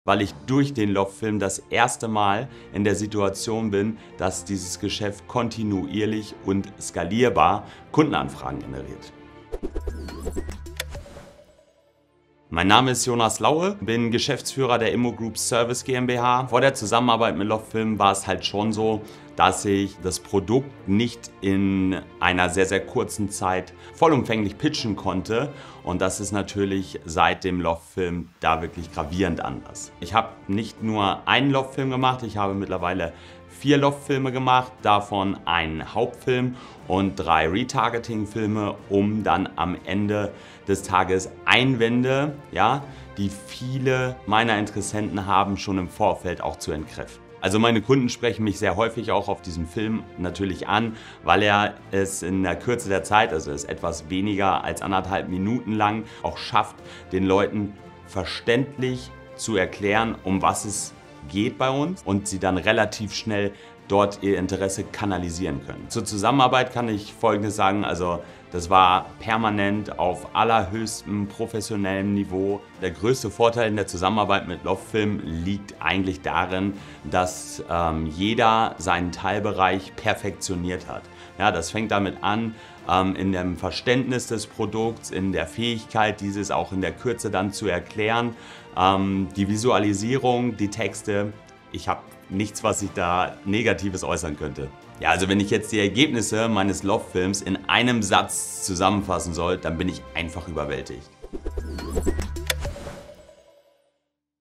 Durch den Loft Film erhalten wir regelmäßig Kundenanfragen - Erfahrungen mit Loft Film
Jonas Laue ist Geschäftsführer der ImmoGroup Service GmbH.
Sein Angebot: Vermögensaufbau mit Pflegeimmobilie. Das heißt, eine Investition in ein Appartement einer Pflegeeinrichtung.

Dieses Angebot ist jedoch umfangreich und schwer in kurzer Zeit zu erklären.

Jonas hat bereits 4 Loft Filme produzieren lassen. Er setzt seine Erklärfilme unter anderem ein, um Einwände seiner Interessenten schon im Vorfeld zu klären.

Durch den Einsatz seines Loft Films bekommt Jonas kontinuierlich Kundenanfragen, die dadurch skalierbar sind.